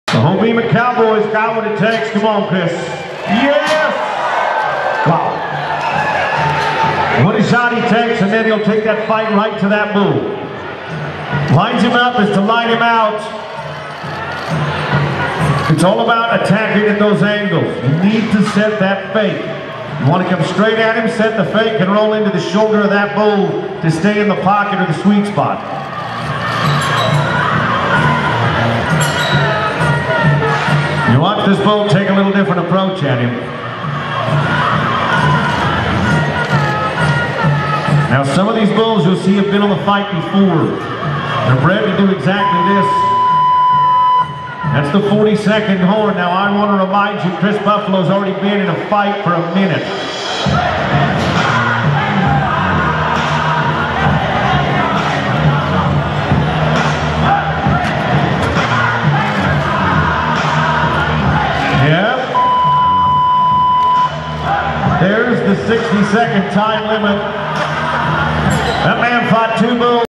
Kris Buffalo gets clipped by a bull
at the ABBI Canada Classic Finals in Ponoka, Alberta on December 31st, 2017.